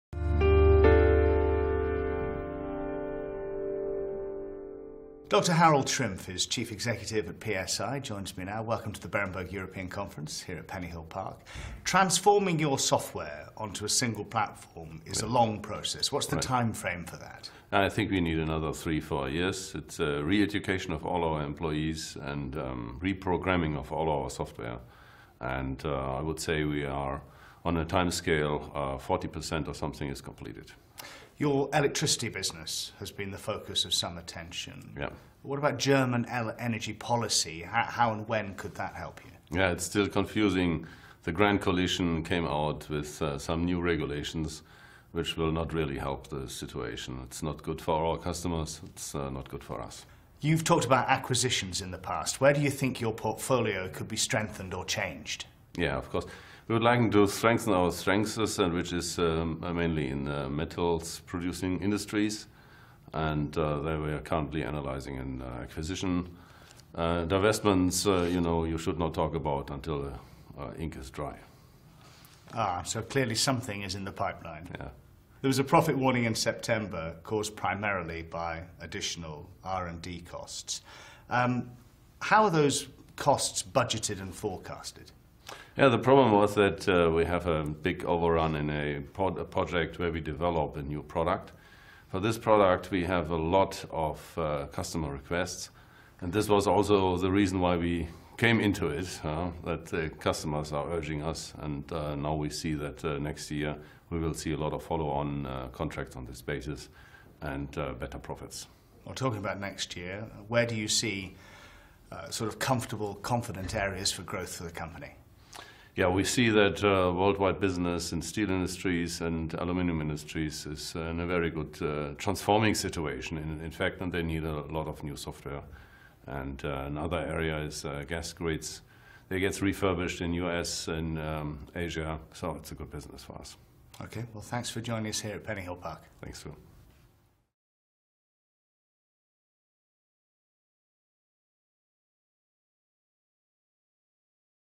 Berenberg European Conference in Penny Hill | PSI AG | 05 December 2013
Berenberg in Corporate Dialogue at the Berenberg European Conference in Penny Hill Park with Harald Schrimpf, CEO of PSI AG.